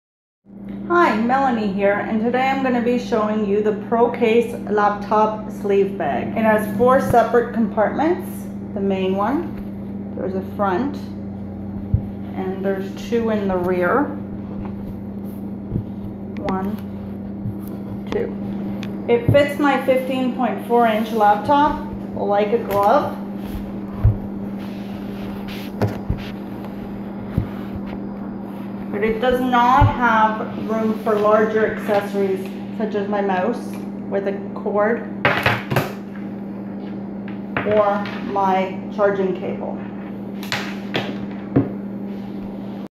Procase 15
This is the video component for my amazon.ca review of the ProCase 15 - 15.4 Inch Felt Laptop Sleeve Bag.